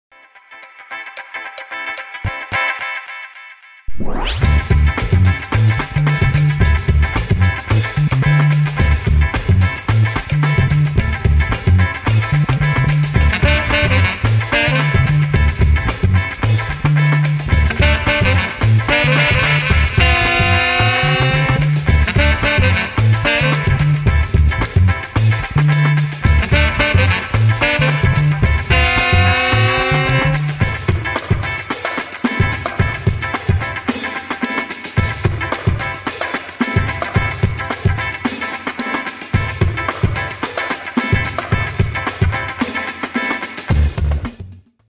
Goody's Burger House @ Burger Fest '17
Τα Goody's Burger House θα είναι στο Burger Fest 2017, το μεγαλύτερο food festival της Αθήνας αφιερωμένο στα burgers! Σε περιμένουμε από Πέμπτη 18 εώς Κυριακή 21 Μαΐου στην Τεχνόπολη του Δήμου Αθηναίων.